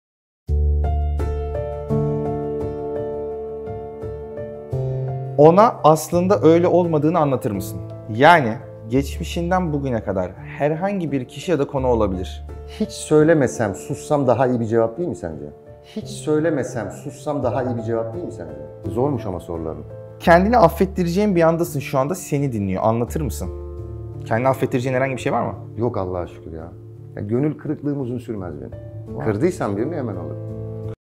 Ahmet Kural’ın Geçmişiyle İlgili O Soruya Verdiği Olay Yanıt!
Müjdemi İsterim filmi başrolü Ahmet Kural, filmle ve kendi hayatıyla ilgili çarpıcı yanıtlar verdi.